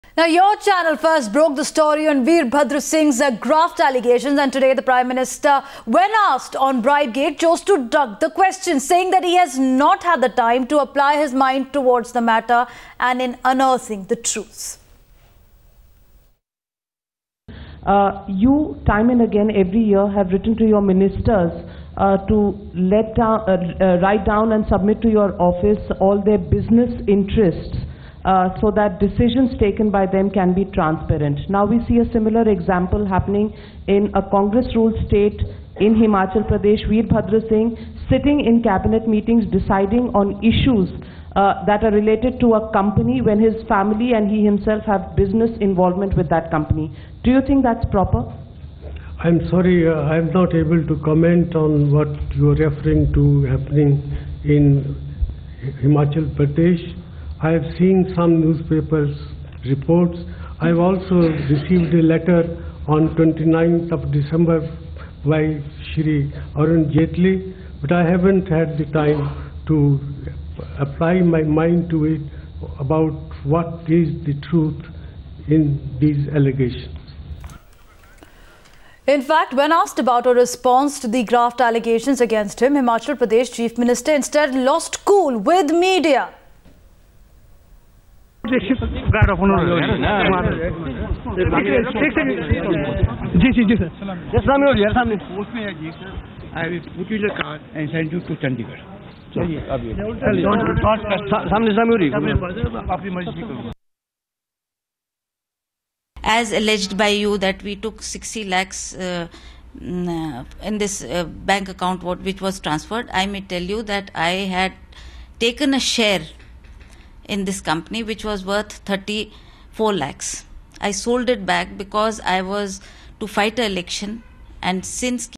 Cornered Virbhadra targets media
TIMES NOW first broke the story on Virbhadra Singh's graft allegations and today Prime Minister Manmohan Singh when asked on Bribegate chose to duck the question - saying that he has not had time to apply his mind towards the matter and in unearthing the truth. Download the Times Now India's Election HQ app and get all the election info at one go. Social Media Links